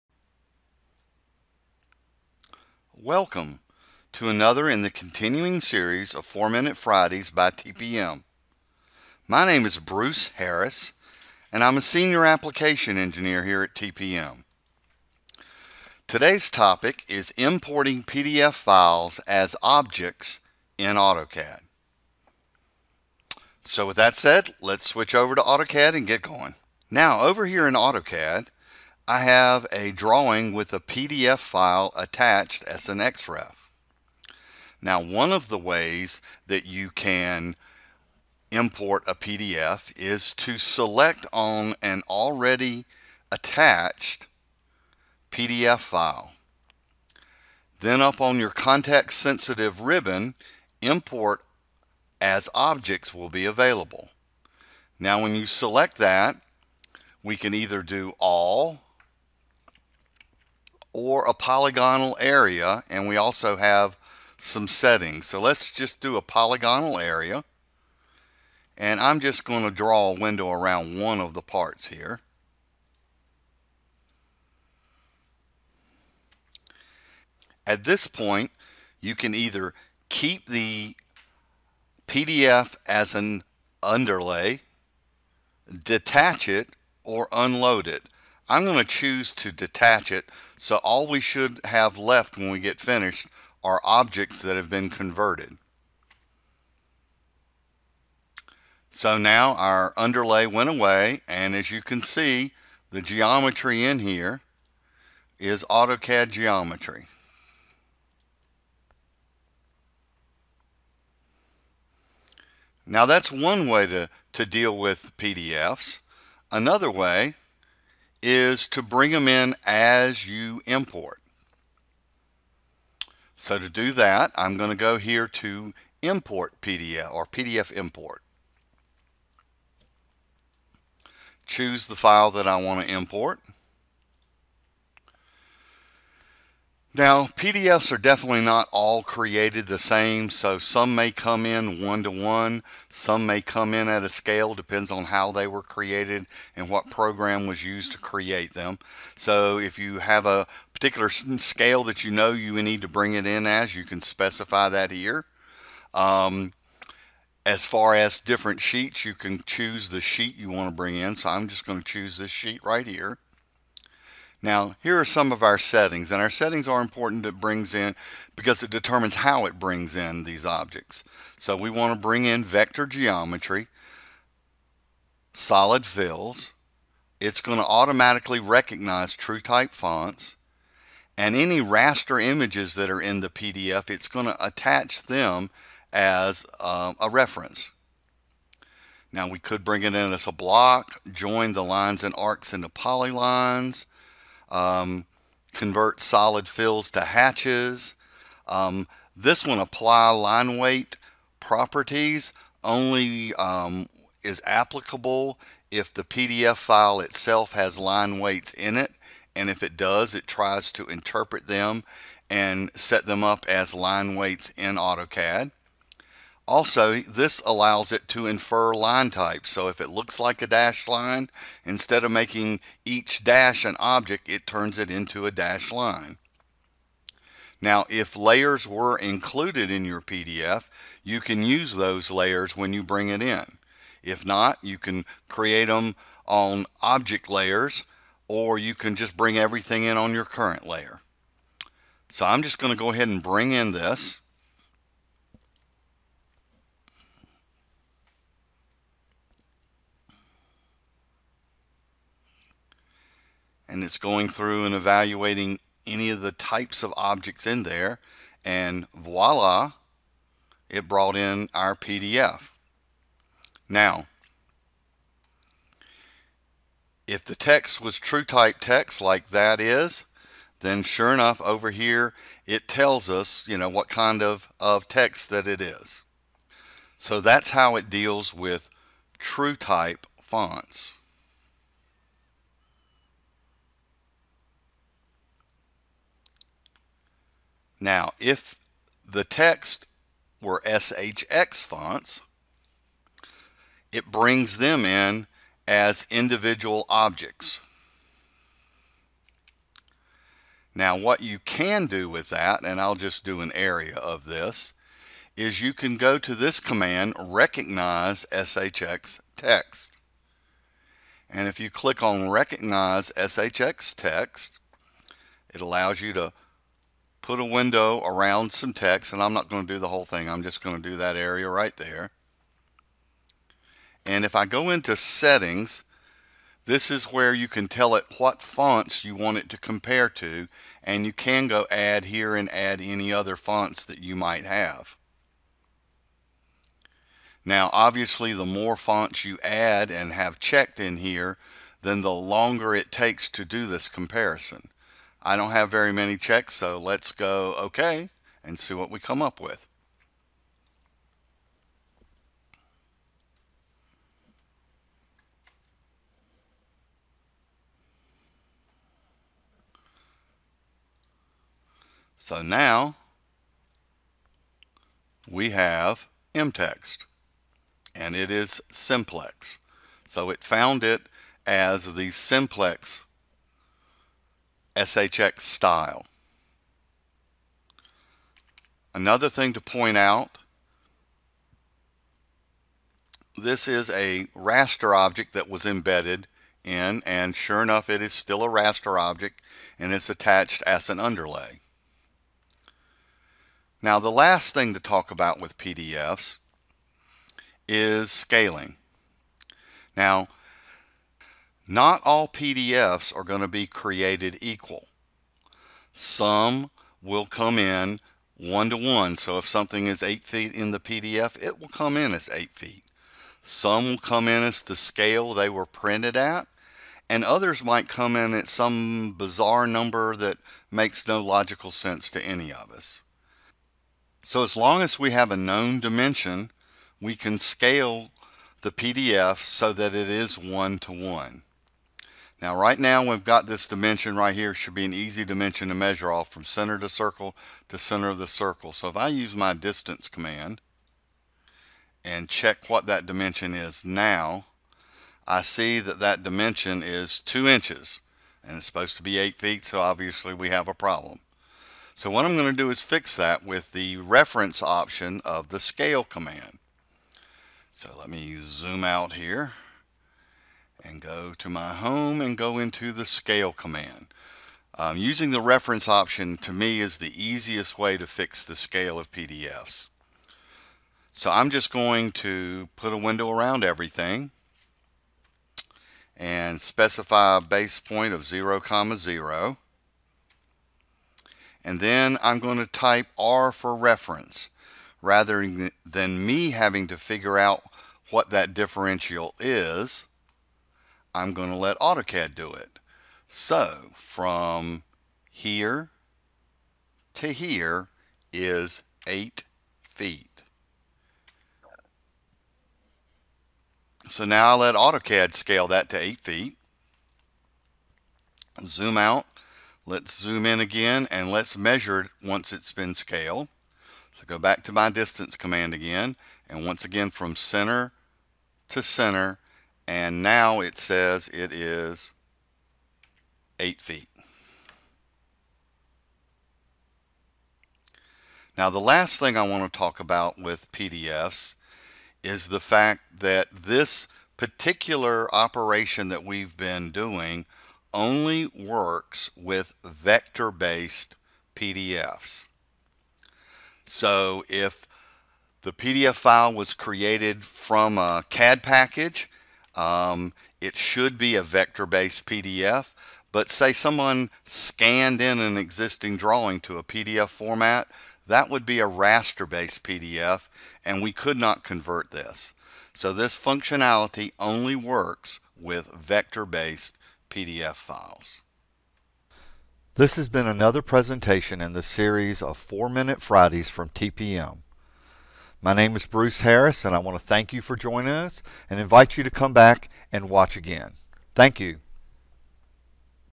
Importing PDF files as objects in AutoCAD
This Four minute Friday shows how to import PDF files as objects in AutoCAD.